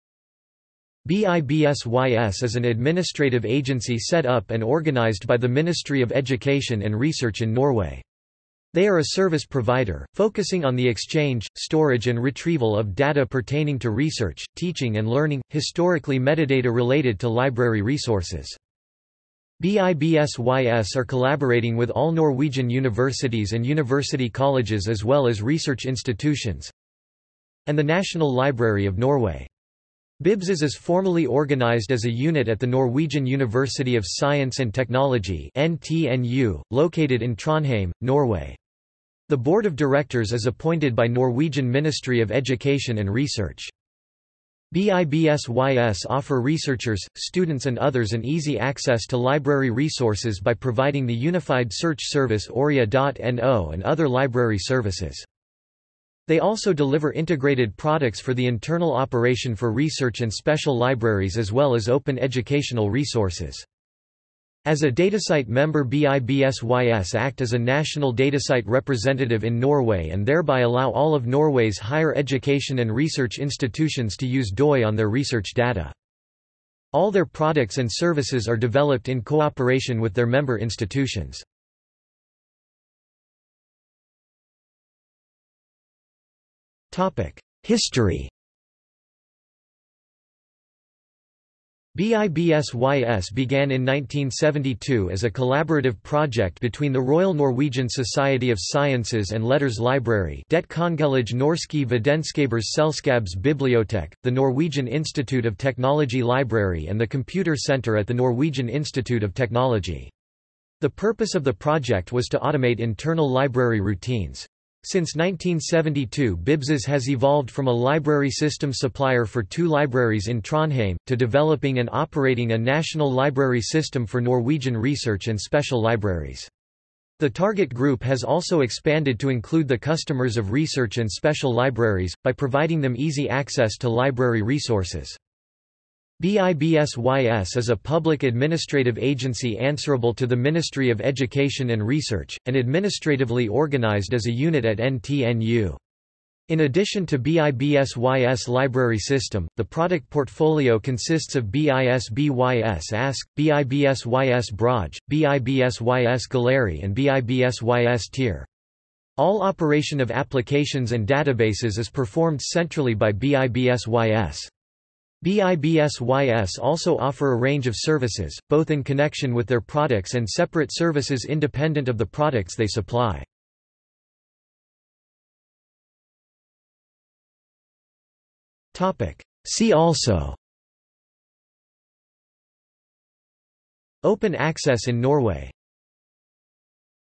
Bibsys | Wikipedia audio article
This is an audio version of the Wikipedia Article:
Bibsys

00:01:23 1 History
00:02:55 2 See also

- Socrates

SUMMARY
BIBSYS is an administrative agency set up and organized by the Ministry of Education and Research in Norway. They are a service provider, focusing on the exchange, storage and retrieval of data pertaining to research, teaching and learning – historically metadata related to library resources.
BIBSYS are collaborating with all Norwegian universities and university colleges as well as research institutions
and the National Library of Norway. Bibsys is formally organized as a unit at the Norwegian University of Science and Technology (NTNU), located in Trondheim, Norway. The board of directors is appointed by Norwegian Ministry of Education and Research.
BIBSYS offer researchers, students and others an easy access to library resources by providing the unified search service Oria.no and other library services.
 They also deliver integrated products for the internal operation for research and special libraries as well as open educational resources.
As a DataCite member BIBSYS act as a national DataCite representative in Norway and thereby allow all of Norway's higher education and research institutions to use DOI on their research data.
All their products and services are developed in cooperation with their member institutions.